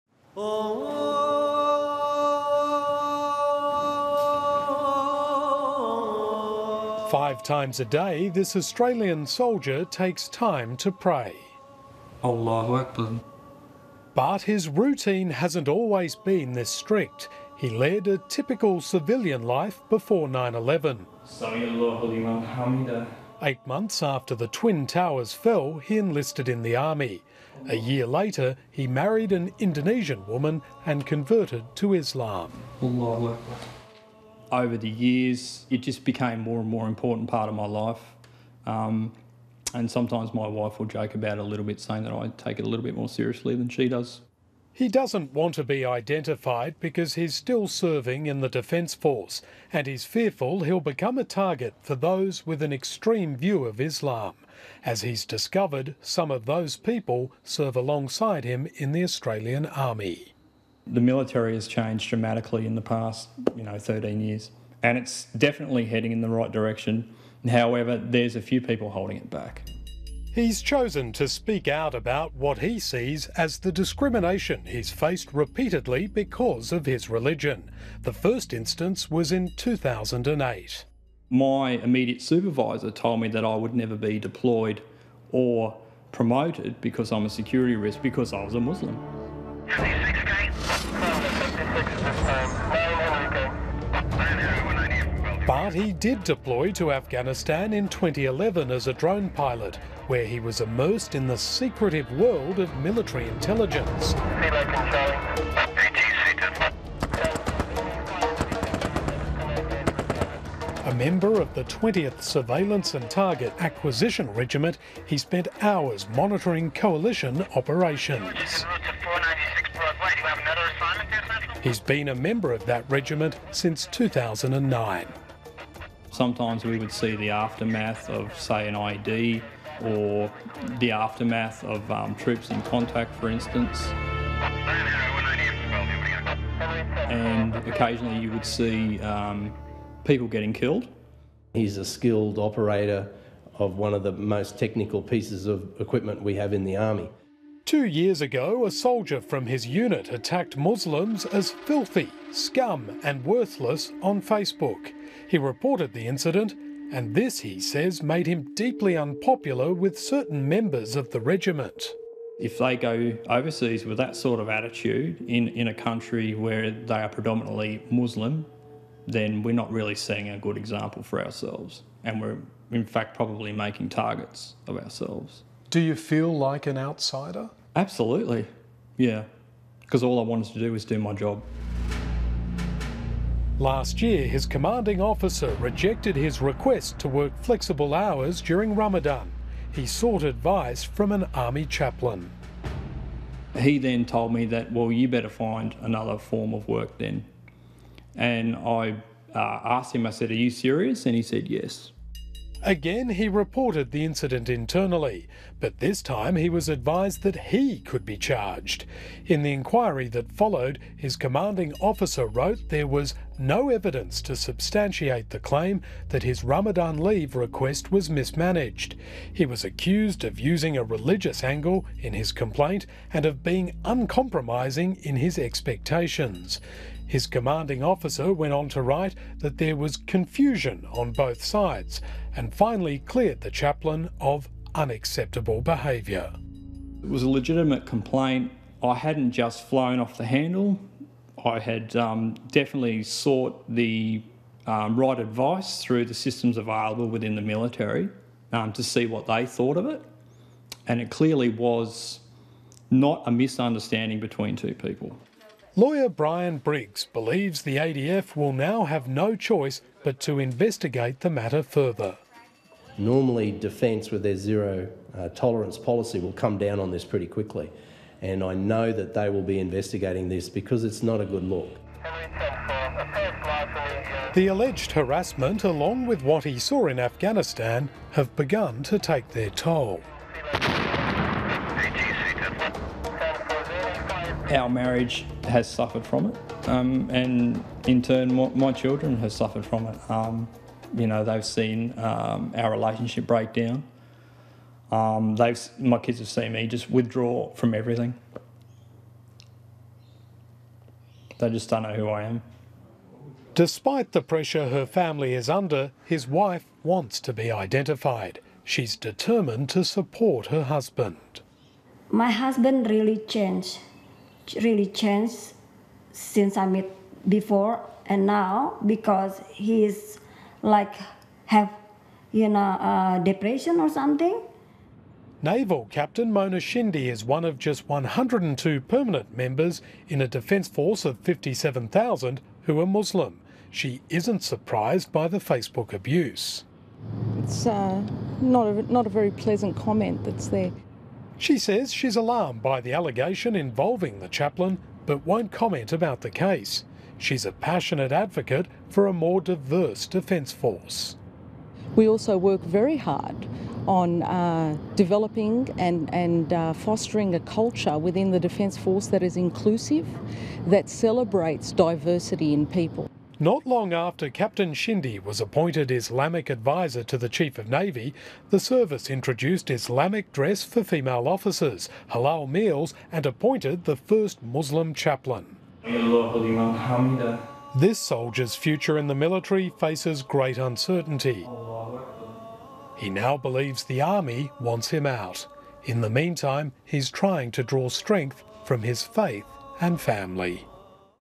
Australian Muslim soldier says he was labelled a 'security risk' and faced discrimination
An Australian soldier says he has faced continued discrimination from the military because of his religion, and that he was labelled a security risk.